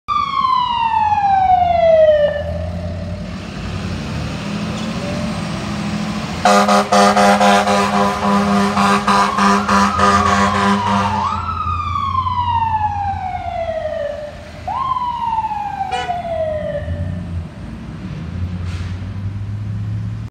MCFRS Medic 703 Responding
Trouble breathing ALS-1/ M703 (AT703 canceled) BOX 3-19
AT703 did not go.